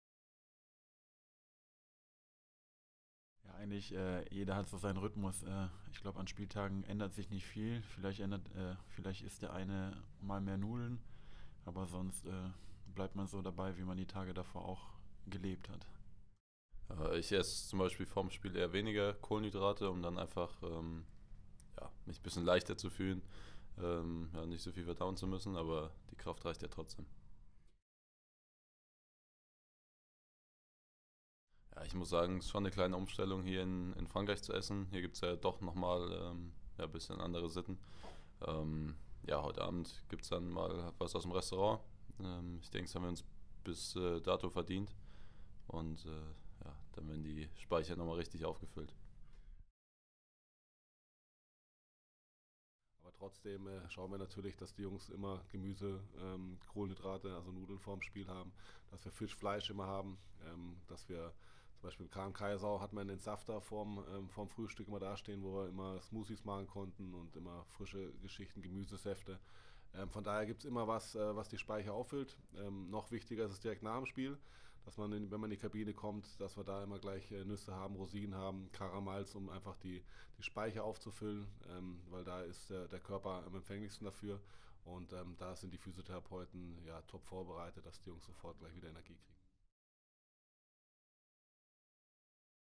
AOK nähergehtnicht: Thema Ernährung
Die Kreisläufer Patrick Wiencek und Jannik Kohlbacher sowie Teammanager Oliver Roggisch sprechen über die Nahrungsaufnahme vor und während der WM in Frankreich